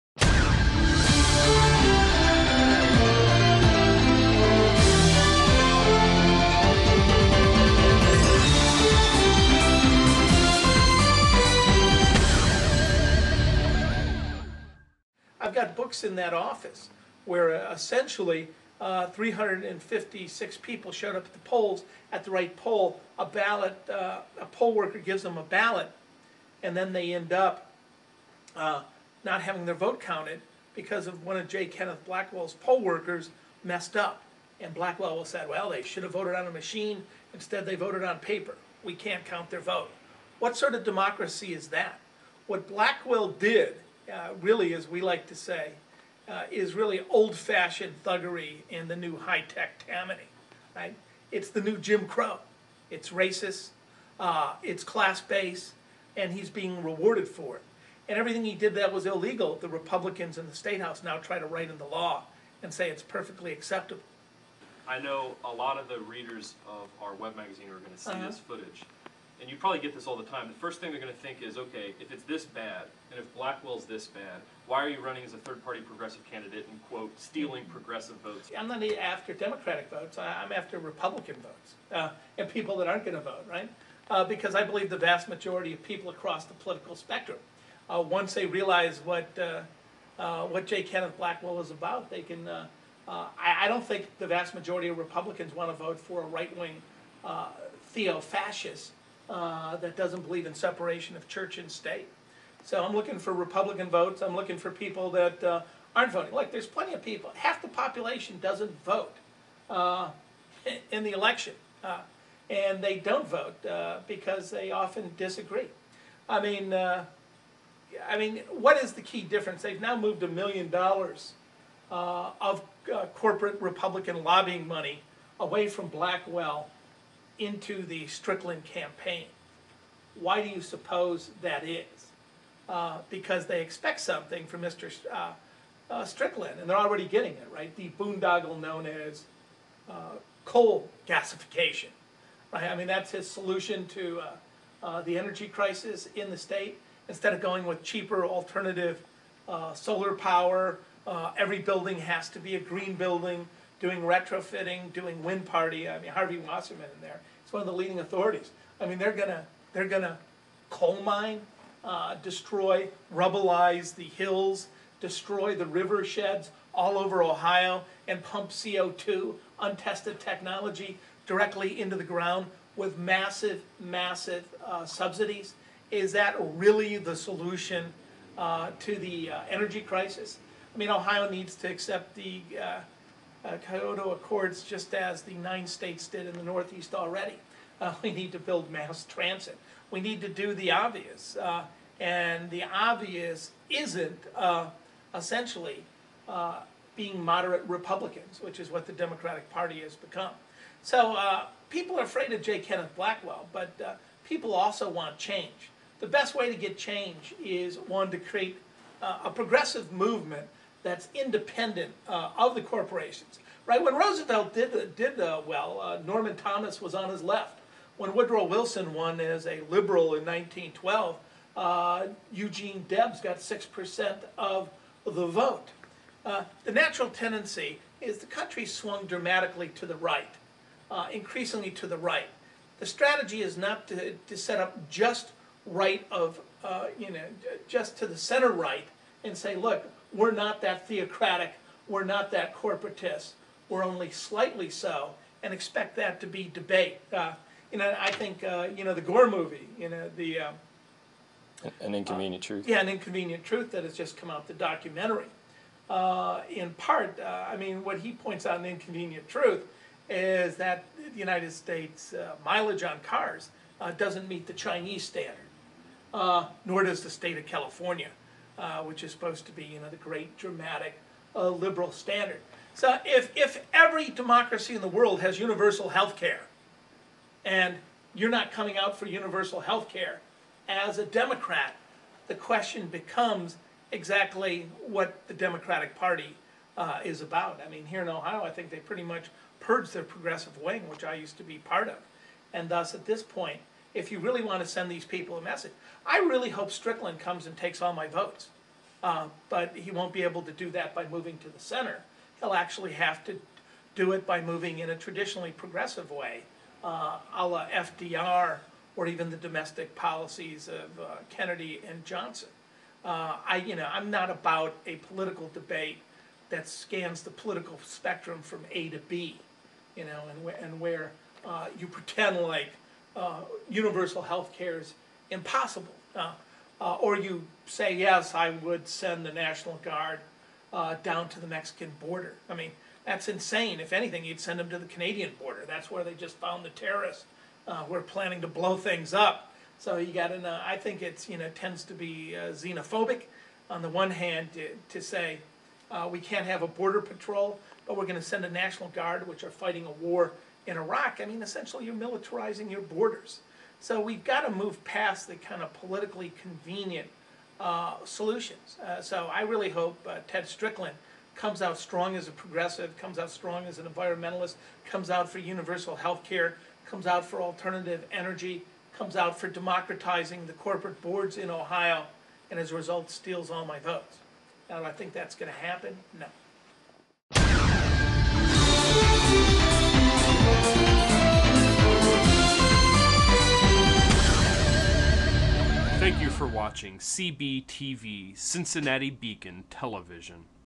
The Fitrakis Files, Part Three: Stealing Progressive Votes?
Dr. Bob Fitrakis discussing why he is running for Governor in Ohio on the Green ticket in 2006.